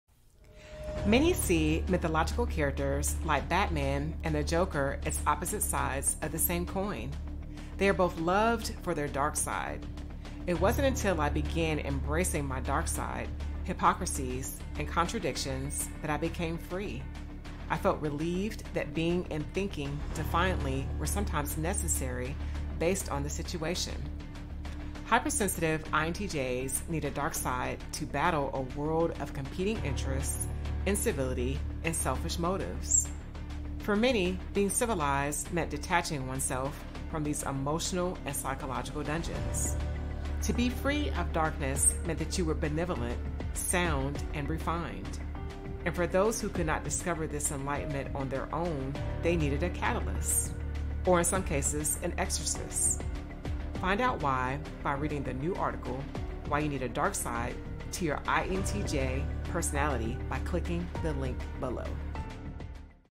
Why You Need a Dark Side to Your INTJ Personality
Many see mythological characters like Batman and The Joker as opposite sides of the same coin. They are both loved for their dark side.

It wasn’t until I began embracing my dark side, hypocrisies, and contradictions that I became free. I felt relieved that being and thinking defiantly were sometimes necessary based on the situation.

Hypersensitive INTJs need a dark side to battle a world of competing interests, incivility, and selfish motives.